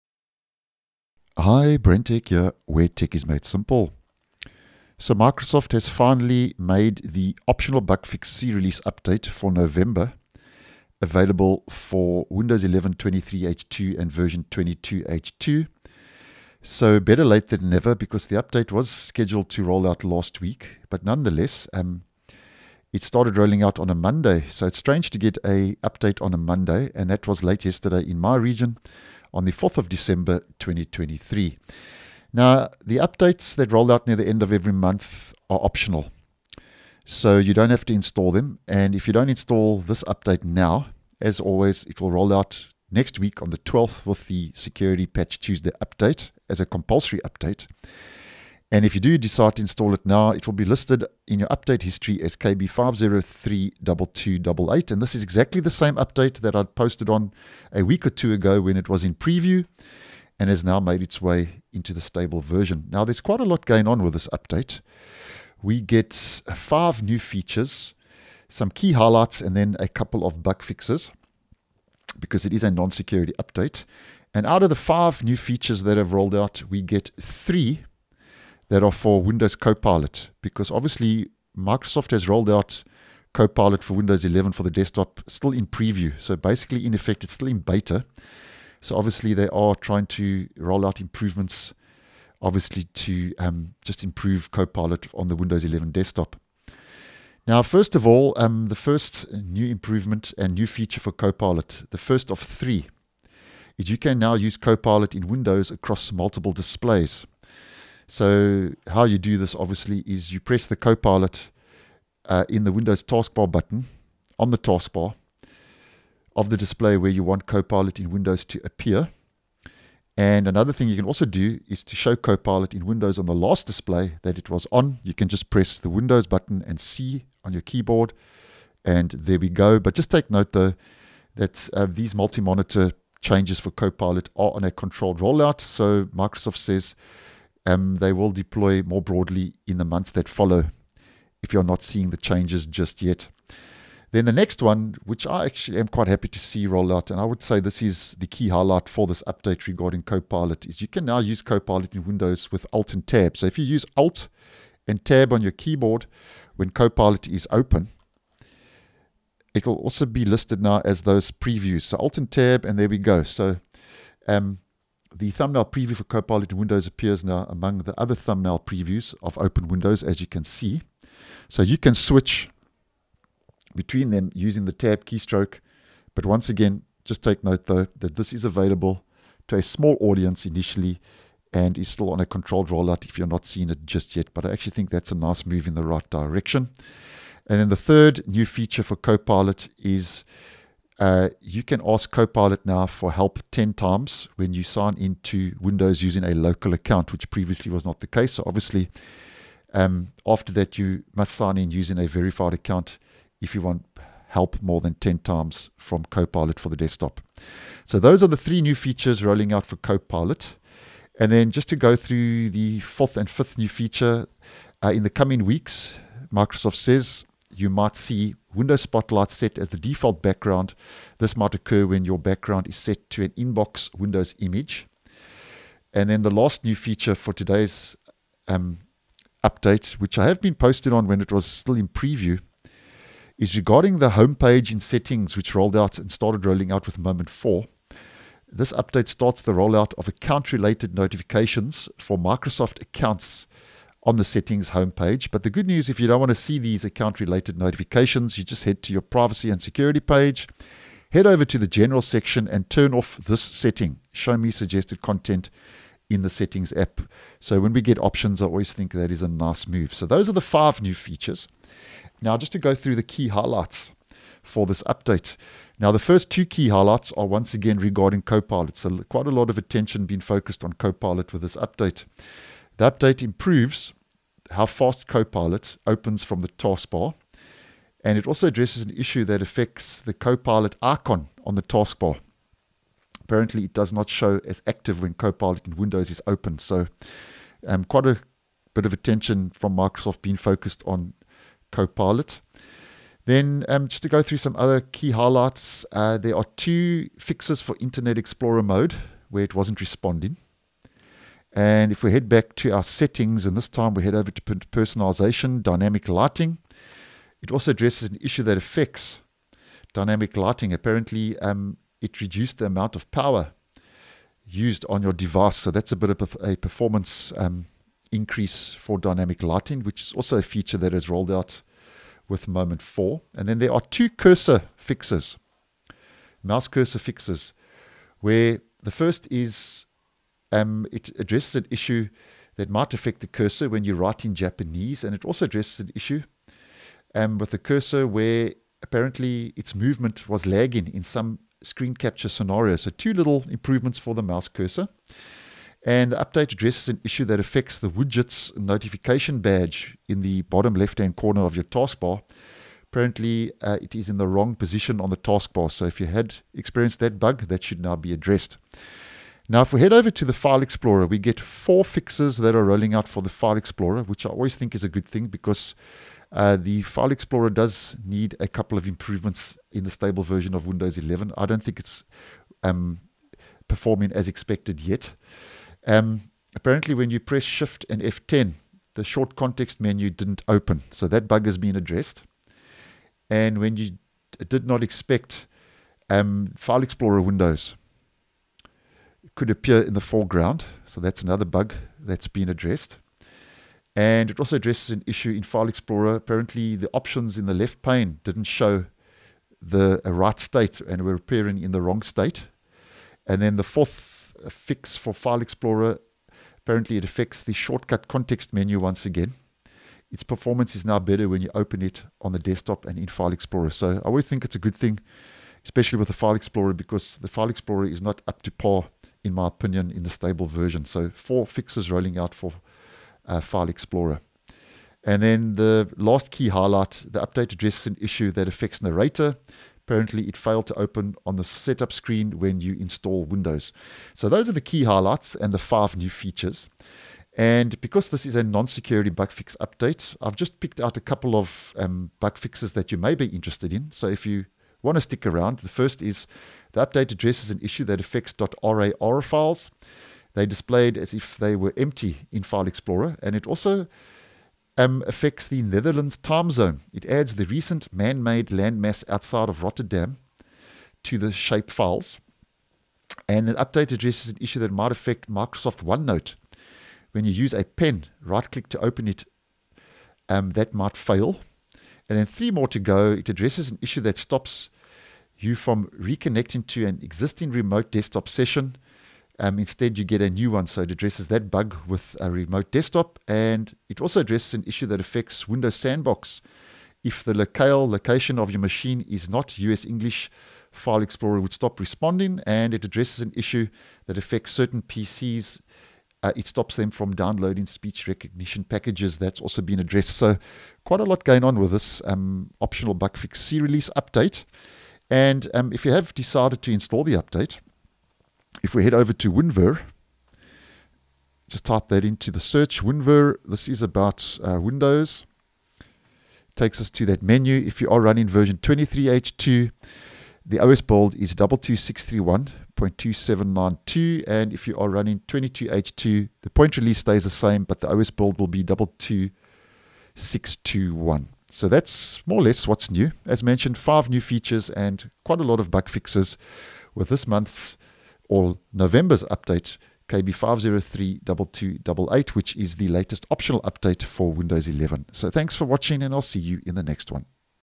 Windows 11 KB5032288 Released with 5 New Features ( 3 for Copilot), Explorer Fixes & More
This latest optional update for November 2023, adds the ability to use Copilot in Windows across multiple displays, you can use Copilot in Windows with ALT +TAB, you can use Copilot ten times when you sign in to Windows using a local account, Windows Spotlight is set as the default background, starts the roll out of account-related notifications and plenty more bug fixes. The update pushes version 22H2 to Build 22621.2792 and version 23H2 to Build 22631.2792.